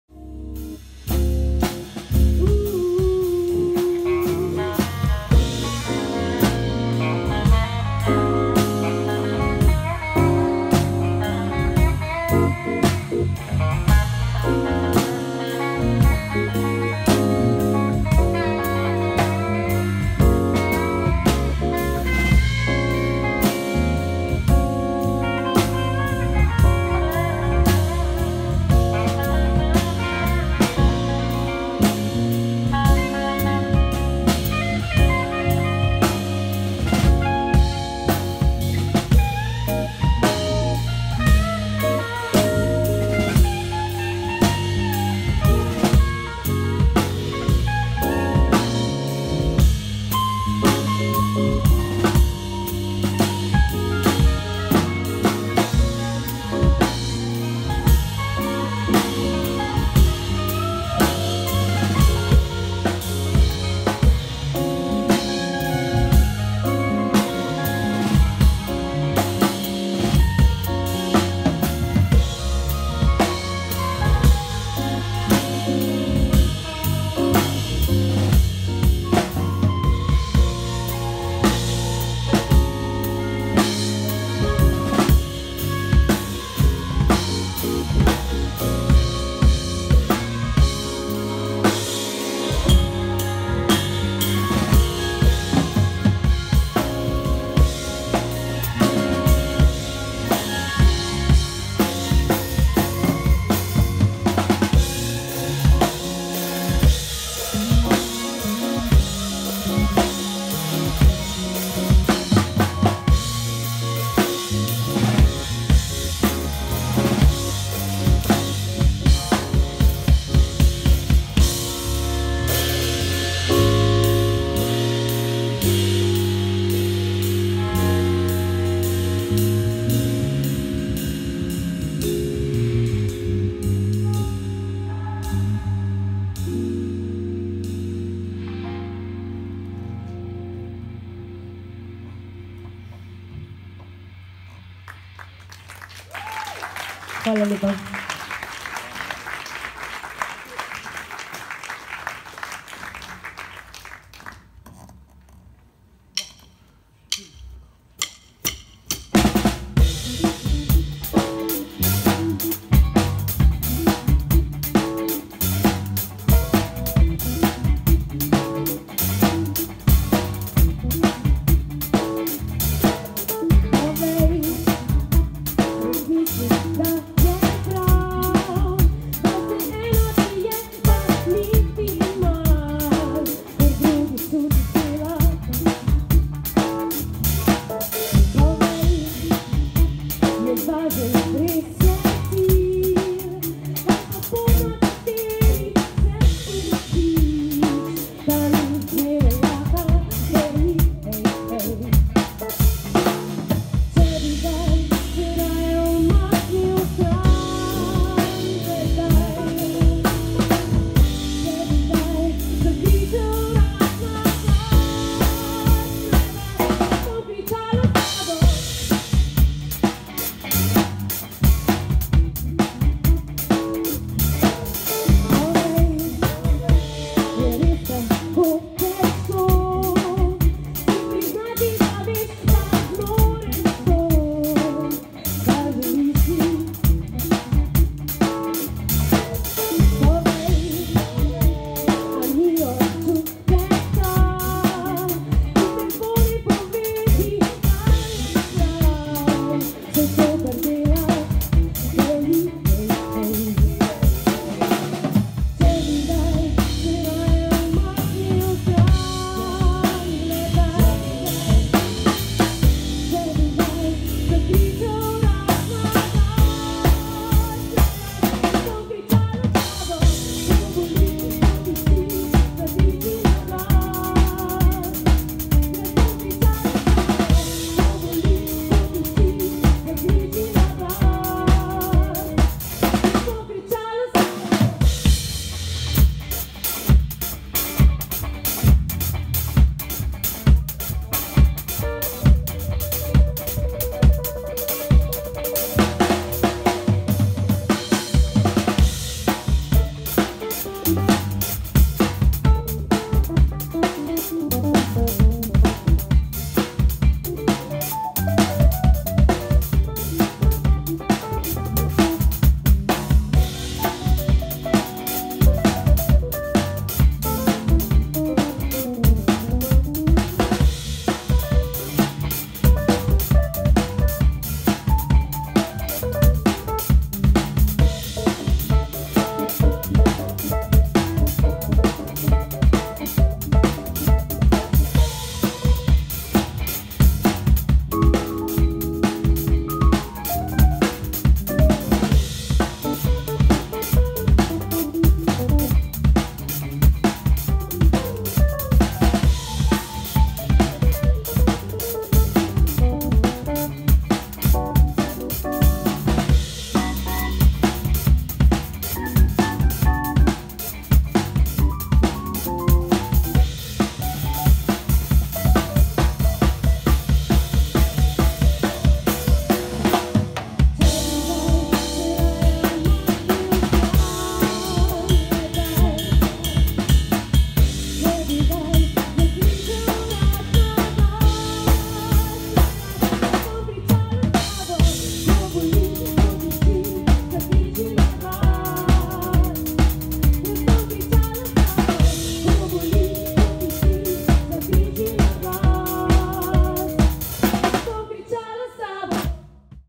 MOJCA live - Jamm and Funky stuff
MOJCA live in Murska Sobota.

Band : Mojca Prejac - vocal, Domen Gnezda - guitar, Robert Ožinger - electric piano, Luka Herman Gaiser - bass, Jože Zadravec - drums, Saša Matajič and Nejc Miklošič - backvocals.